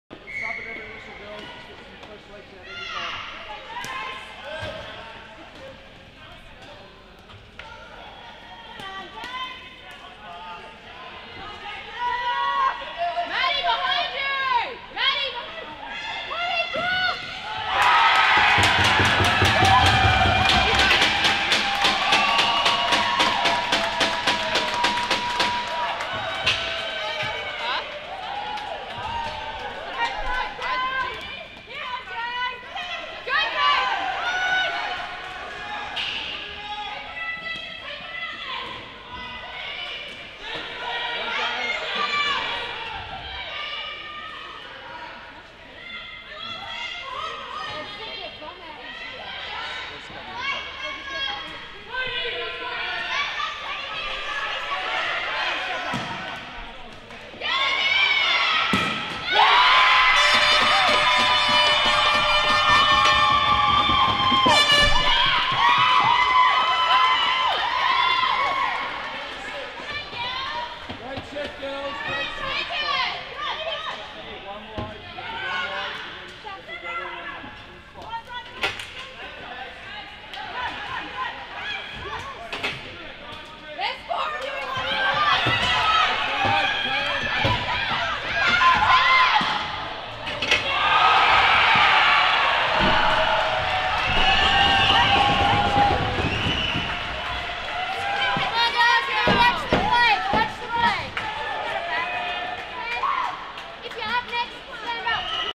Broomball Australia Nationals 2014 - Womens Elite Final - Goals
Goal highlights from the 2014 Australian Broomball Nationals 2014 Womens Elite final.  Held in Canberra, 17-20 April 2014.